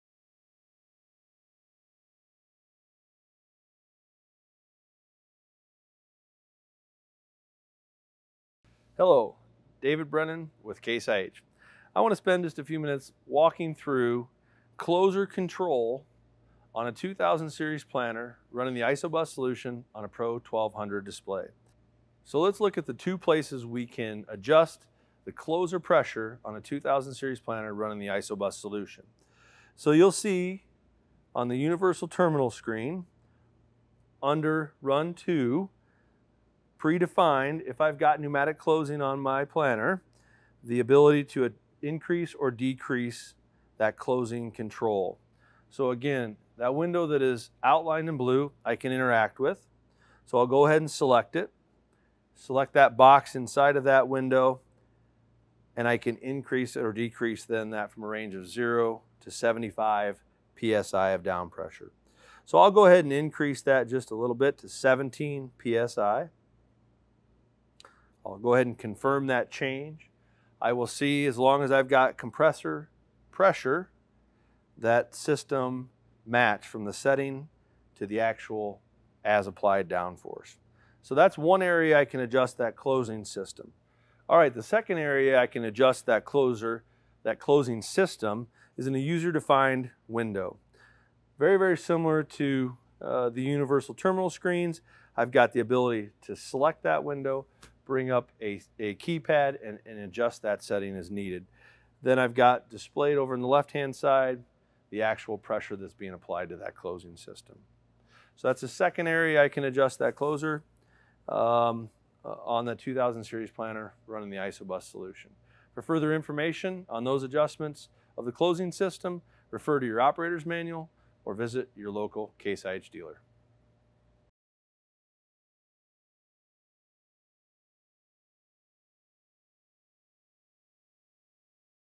Adjustment of the In-Cab Closing System on a 2000 Series Planter using a Pro 1200 Display
In cab closing system adjustment on a 2000 Series Early Riser planter when connected to an AFS Pro 1200 display